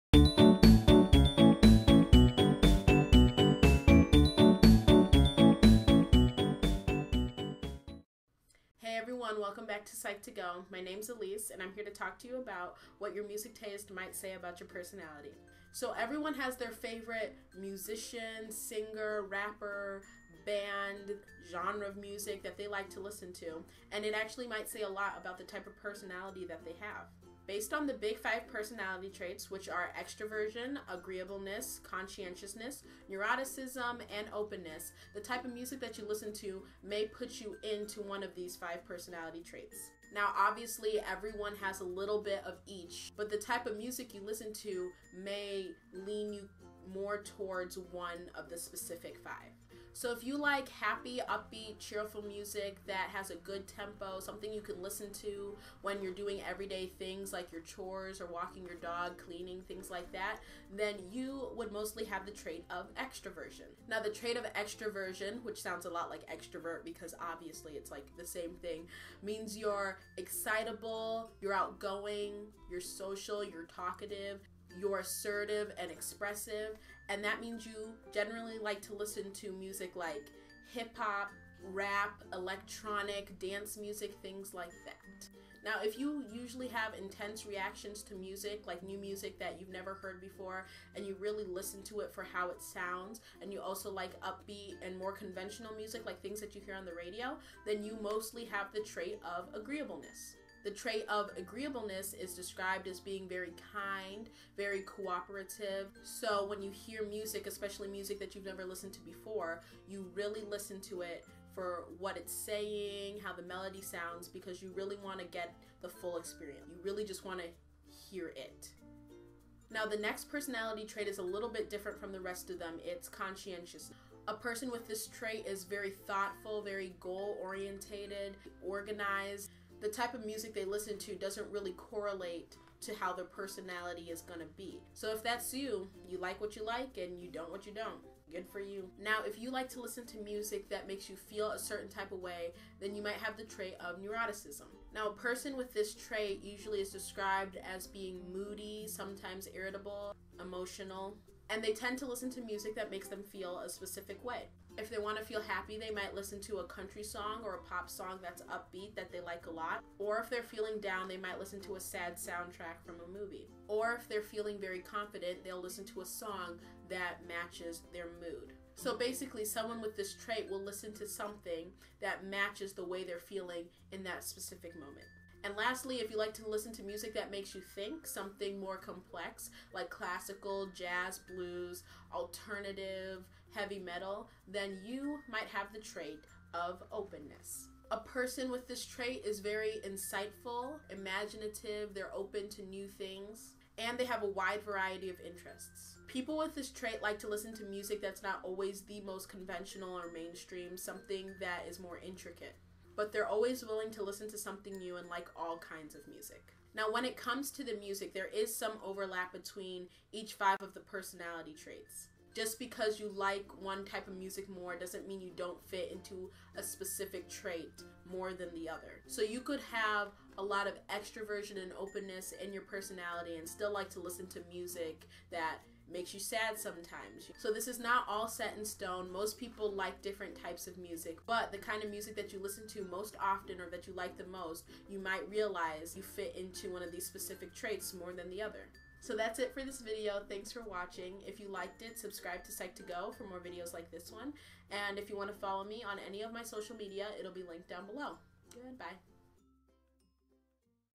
What Music Says About Your Personality | ft. peaceofelise
What personality type do you best fit?  Also, what if your favorite genre of music?